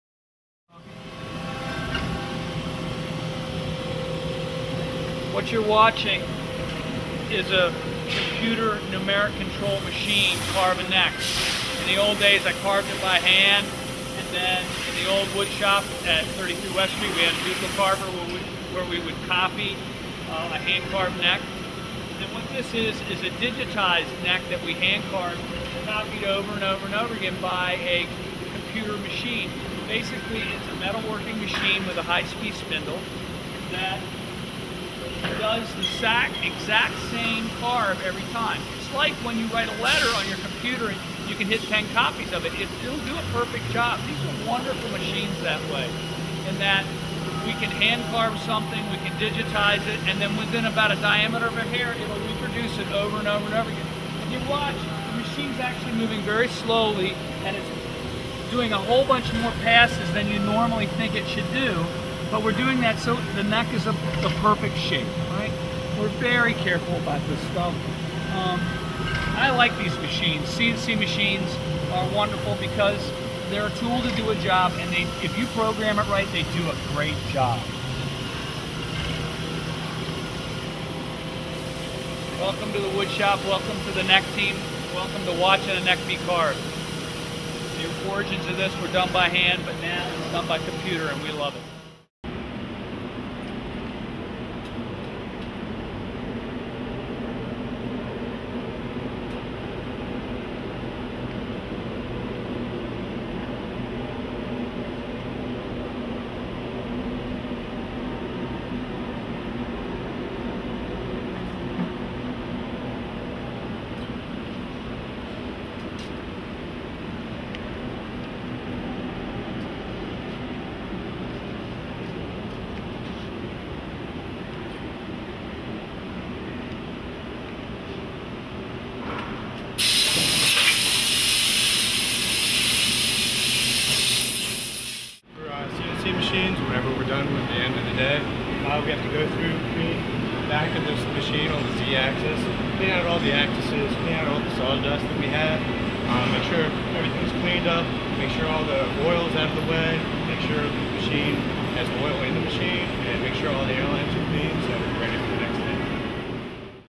CNC Machines & Tooling - Wood Shop - Neck Team
Paul Reed Smith on the history of wood carving at PRS from hand-carving to CNC machines.